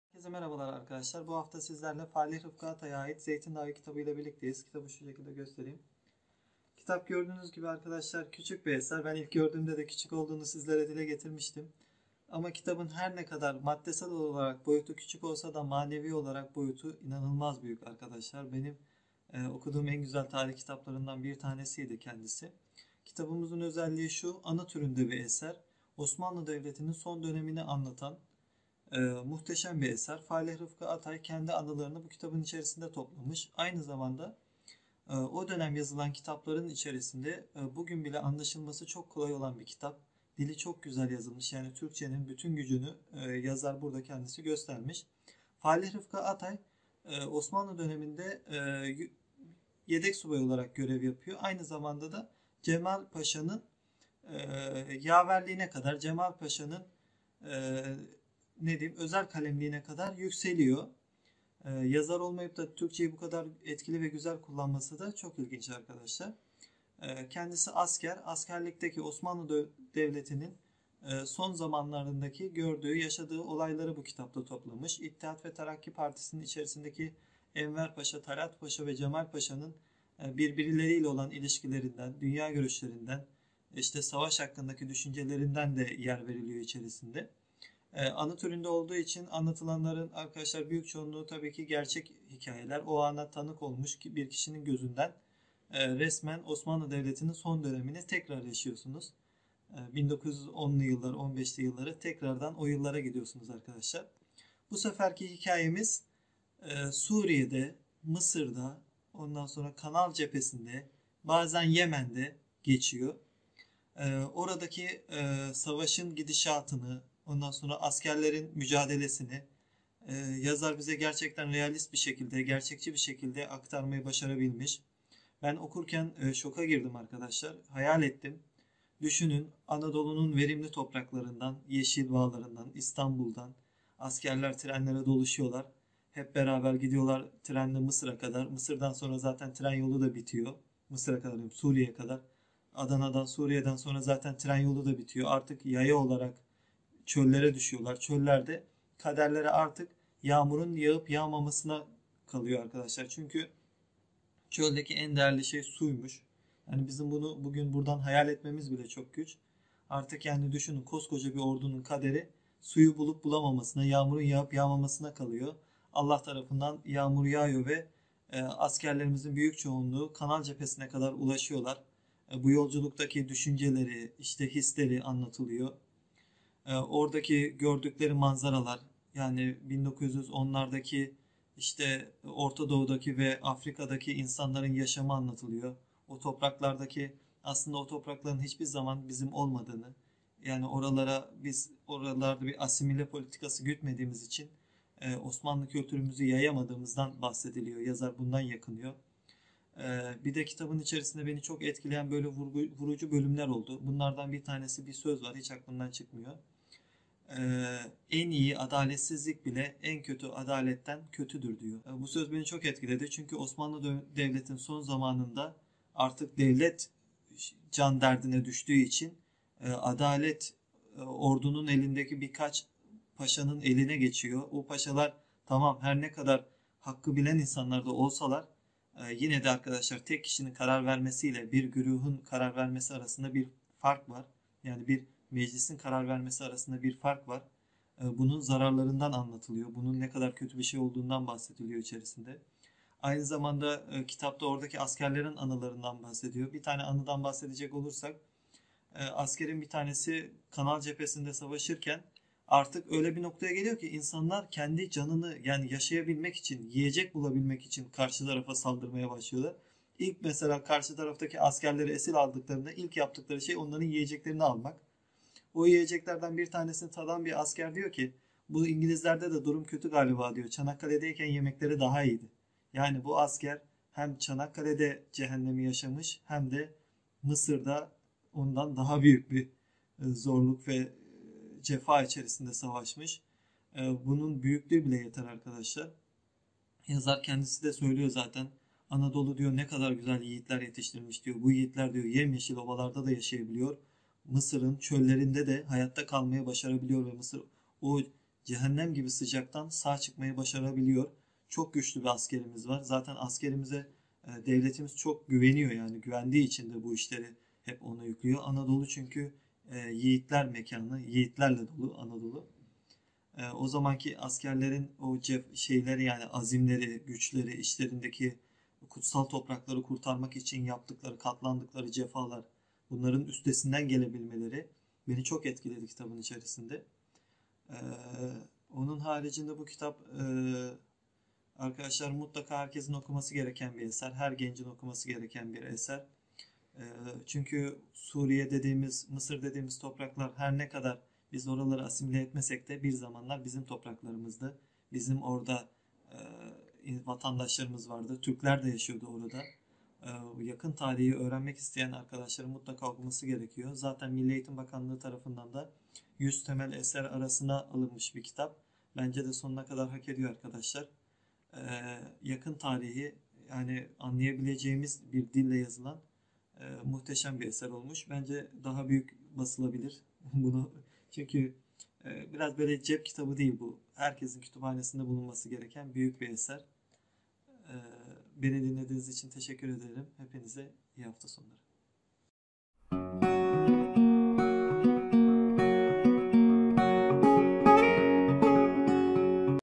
Falih Rıfkı Atay: Zeytindağı
Zeytindağı, Cumhuriyet devri edebiyatının en büyük hadiselerinden biridir.